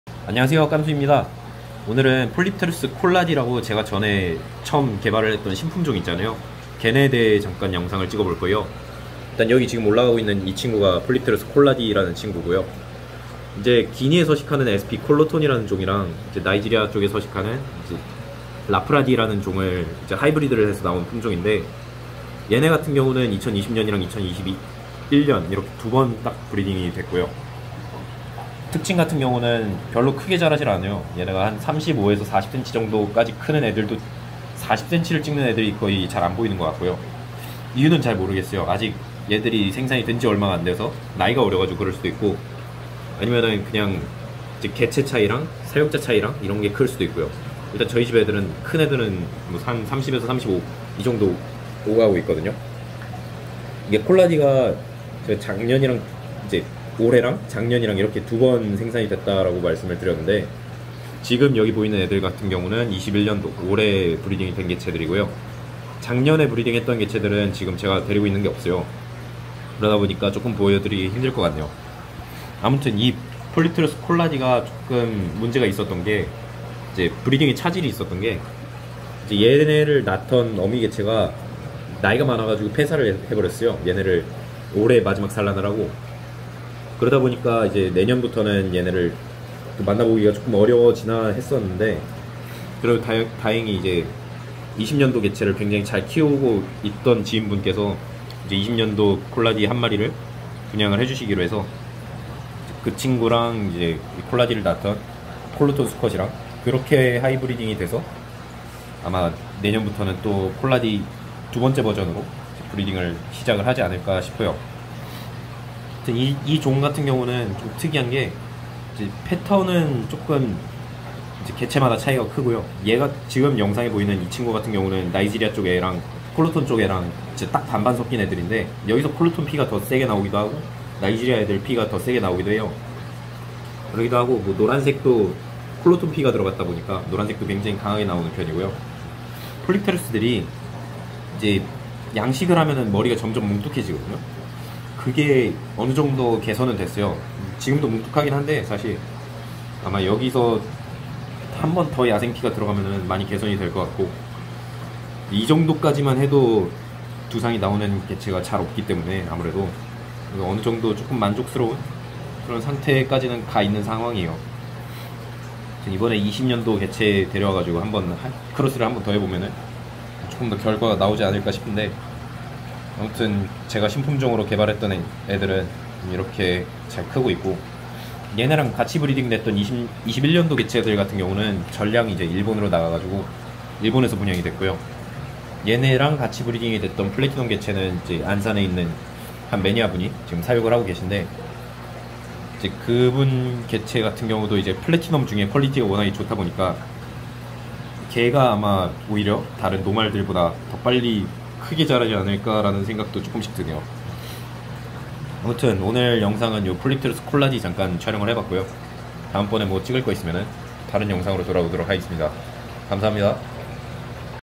제가 개발한 신품종 열대어 근황입니다
오늘 영상은 제가 작년 여름 처음 브리딩에 성공했던 "폴립테루스 콜라디"의 근황 영상입니다.

올해 암컷 종어가 폐사하며 추가 브리딩이 어려운 상황이었는데, 다행이도 2020년도에 브리딩한 개체를 사육중인 매니아분이 개체를 분양해주시기로 하셔서 내년 혹은 내 후년부터 두번째 버젼으로 만나볼 수 있을 것 같습니다.